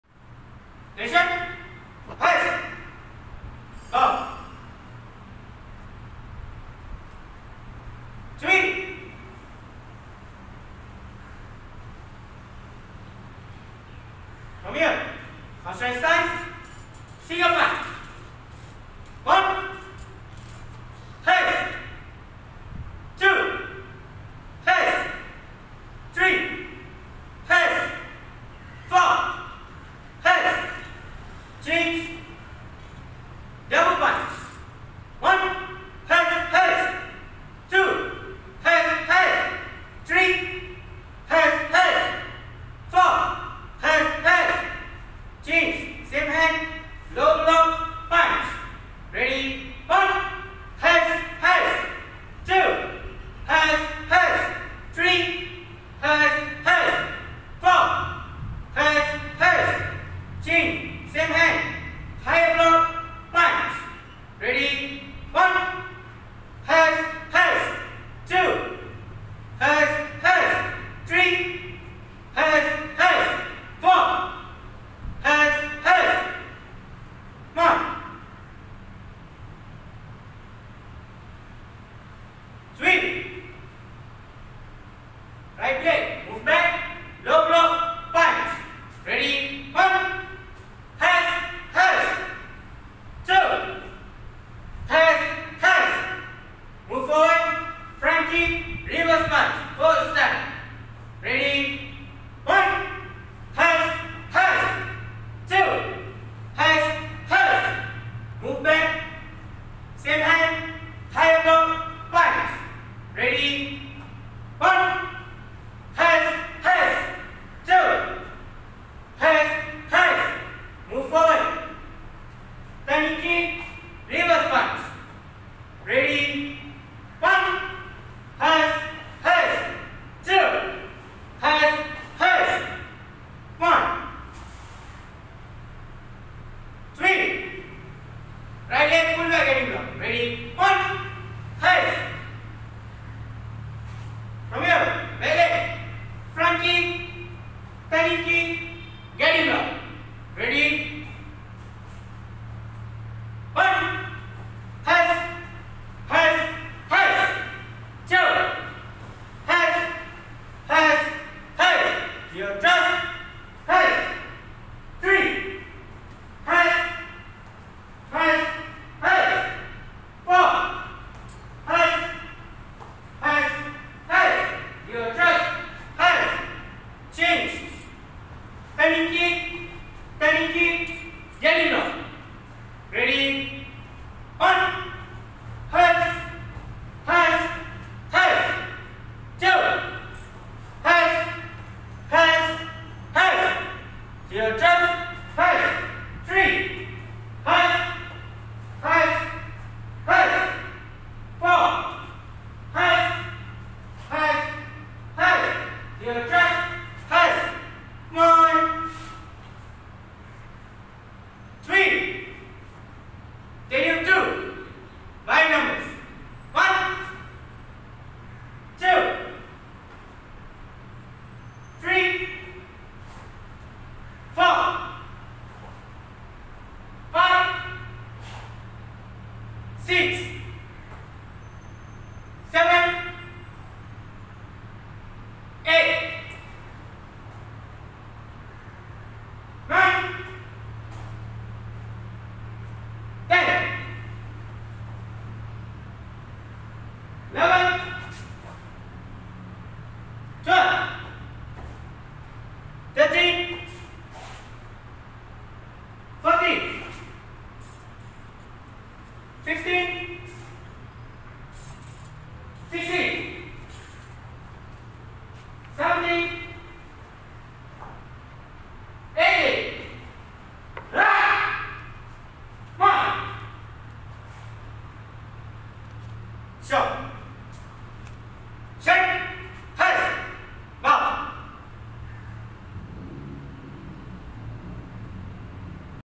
Yellow 2 ( KORYO TAEKWONDO CLUB )
Syllabus pembelajaran tali pinggang kuning 2 KORYO Taekwondo Club
Sylabus ini digunakan untuk pengredan tali kuning 2
Diharapkan pelajar dapat menggunakan video ini sebagai ulang kaji untuk memperbaiki kemahiran masing-masing